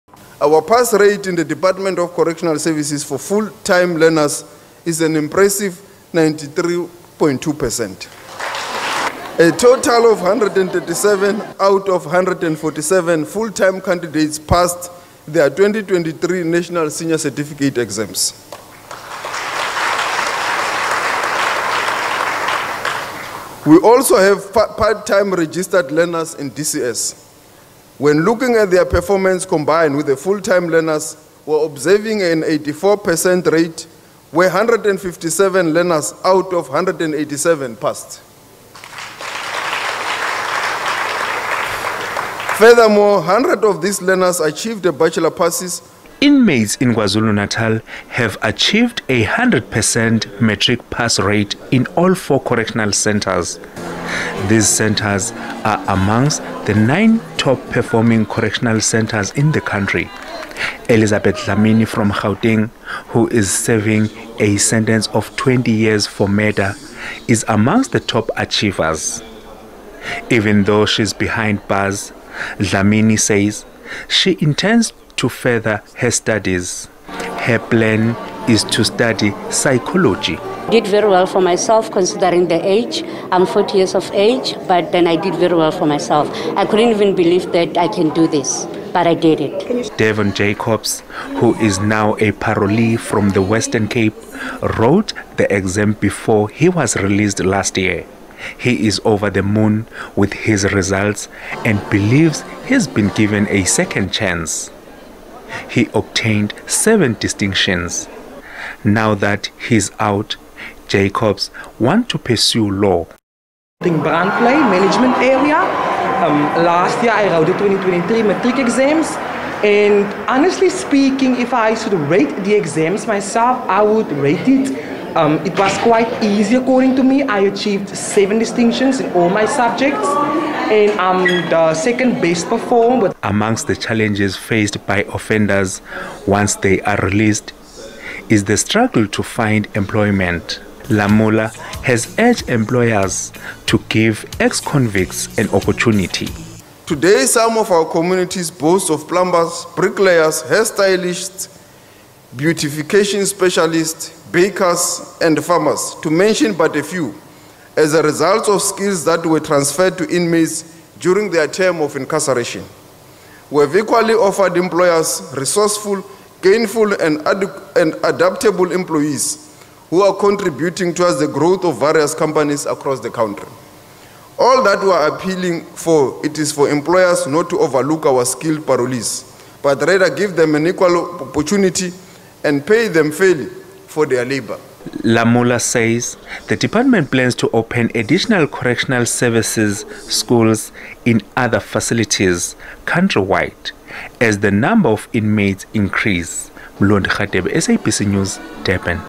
DCS 2023 inmates matric class registered a record breaking 93.2% pass rate
The Department of Correctional Services (DCS) announced the 2023 annual inmates' matric results at the Durban Correctional Centre in Westville on 23 January 2024